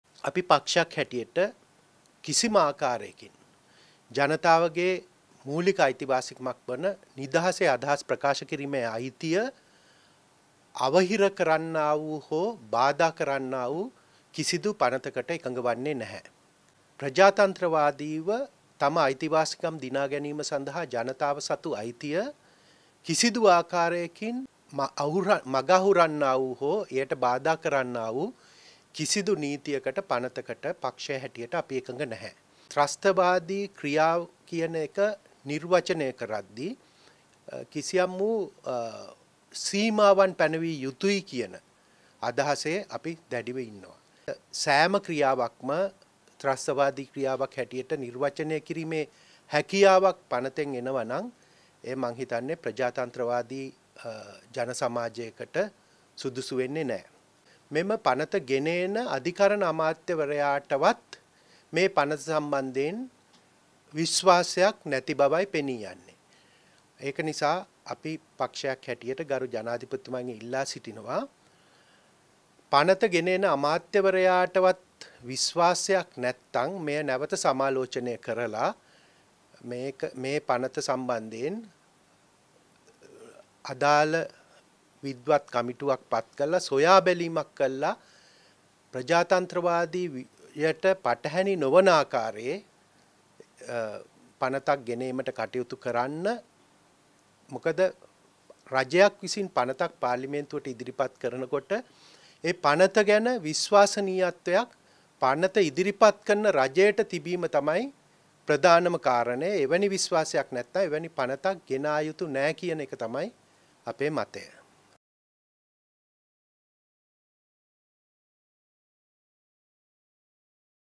විශ්වාසයක් නැත්නම් එවැනි පනතක් ගෙන ආ යුතු නෑ
සාගර කාරියවසම් කියයි